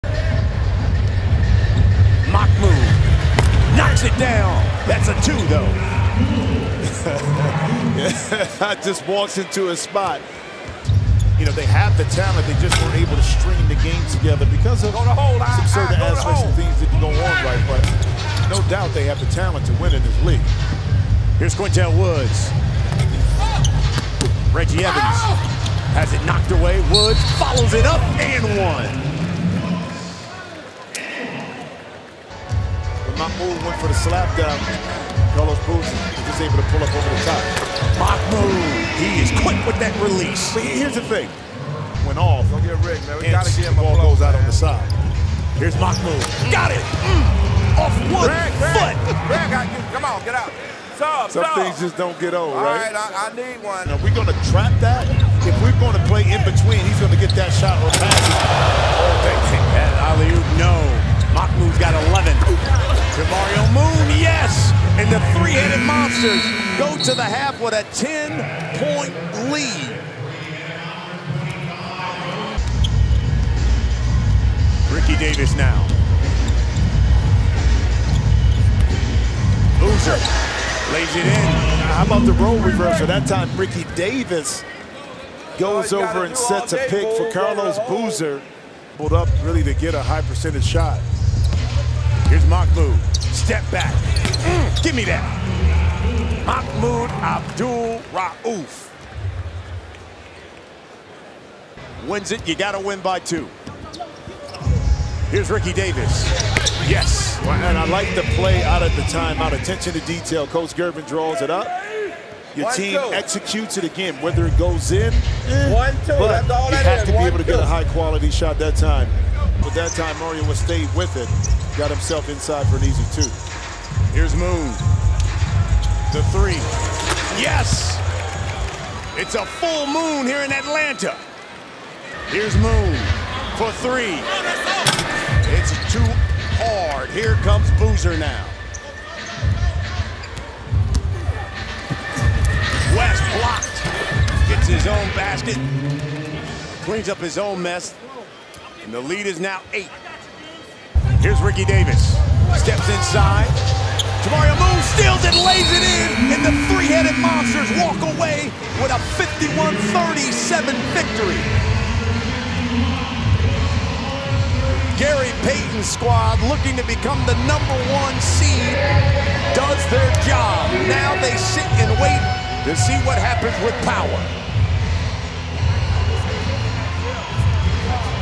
3 Headed Monsters vs Ghost Ballers | BIG3 HIGHLIGHTS
3 Headed Monsters defeat the Ghost Ballers 51-37 in Atlanta to improve to 7-1.